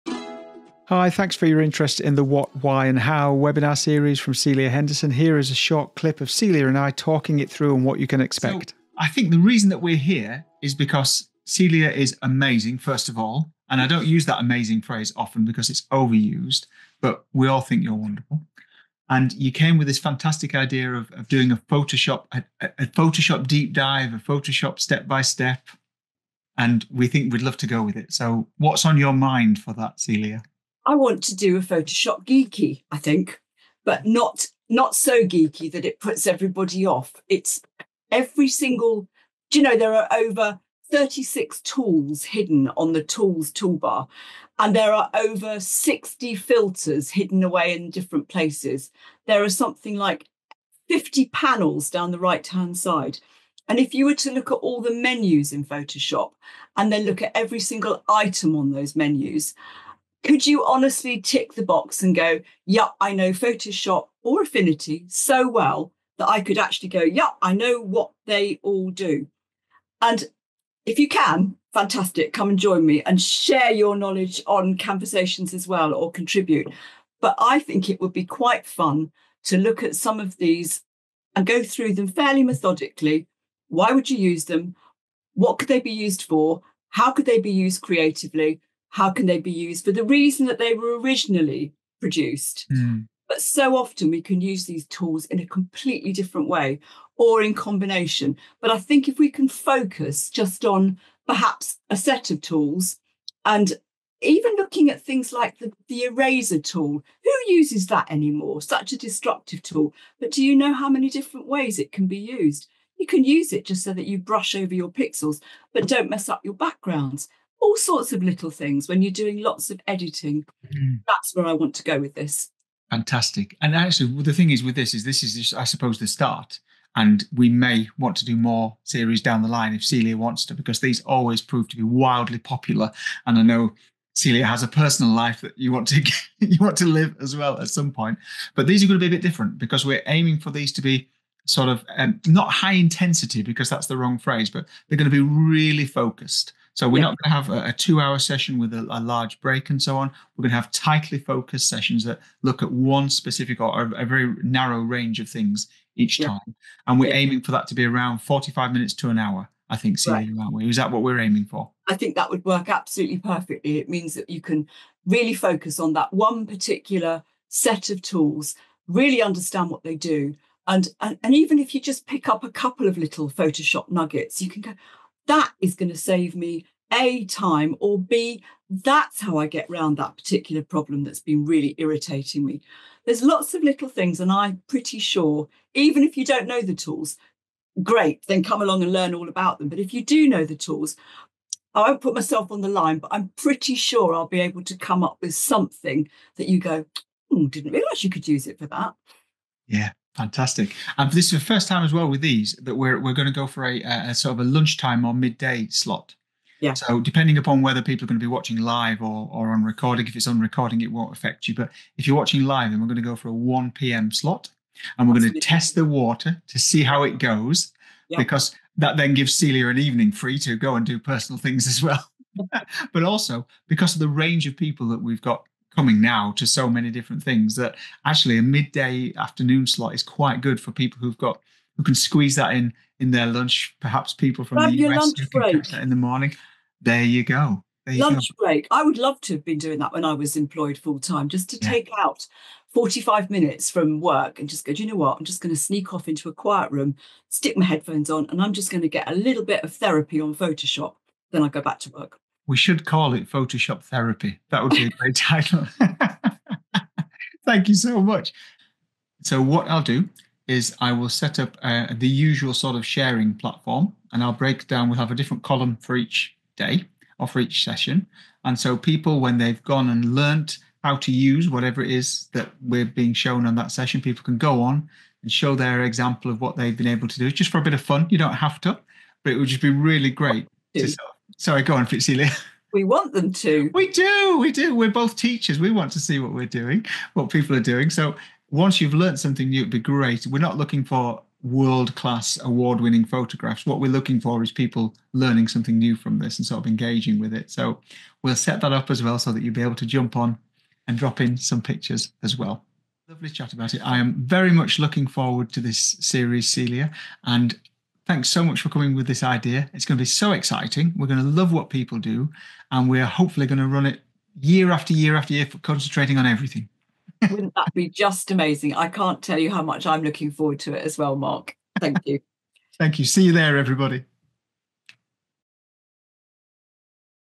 Photoshop Why What How Series Outline
This is a discussion between Celia Henderson and Mark Evans about the upcoming Photoshop workshop 'Why / What / How', wherein Celia will go in to detail about Photoshop and the creative opportunities it offers.

This is an indepth workshop, hosted on our LEARN platform. It will have an optional hand out for each session, along with example images.

All sessions are broadcast live, but are recorded for those who can't make it and are available for viewing within 24hrs.

This series will be available to watch for SIX MONTHS after the final session.

Cost £80 in single payment, or £20 per month for 4 months (+ setup fee).